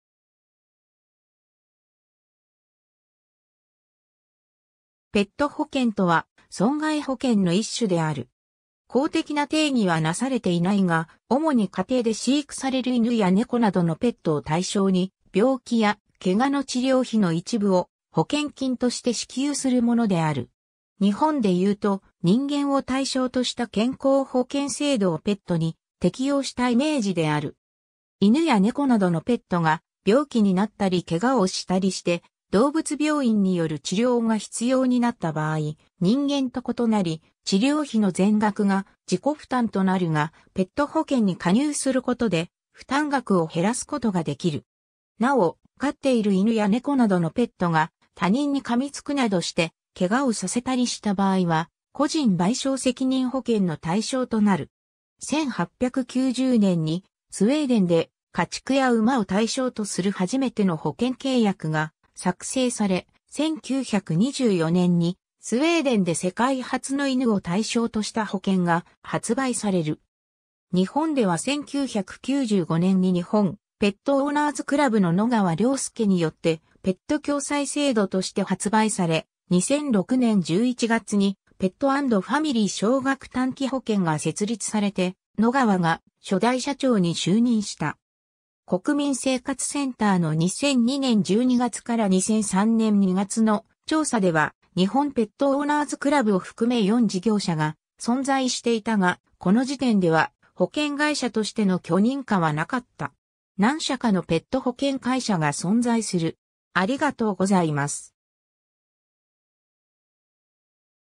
ペット保険
ペット保険（ペットほけん）とは、損害保険の一種である。
公的な定義はなされていないが、主に家庭で飼育されるイヌやネコなどのペット（愛玩動物）を対象に、病気やケガの治療費の一部を保険金として支給するものである。
 日本でいうと、人間を対象とした健康保険制度をペットに適用したイメージである。
 イヌやネコなどのペットが、病気になったりケガをしたりして動物病院（獣医師）による治療が必要になった場合、人間と異なり、治療費の全額が自己負担となるが、ペット保険に加入することで、負担額を減らすことができる。
なお、飼っているイヌやネコなどのペットが他人にかみつくなどしてケガをさせたりした場合は、個人賠償責任保険の対象となる。
 1890年にスウェーデンで家畜や馬を対象とする初めての保険契約が作成され、1924年にスウェーデンで世界初のイヌを対象とした保険が発売される。
 日本では1995年に日本ペットオーナーズクラブの野川亮輔によって「ペット共済制度」として発売され、2006年11月にペット&ファミリー少額短期保険が設立されて、野川が初代社長に就任した。
 国民生活センターの 2002年12月 - 2003年2月の調査では、日本ペットオーナーズクラブを含め4事業者が存在していたが、この時点では保険会社としての許認可はなかった。
 何社かのペット保険（少額短期保険含む）会社が存在する。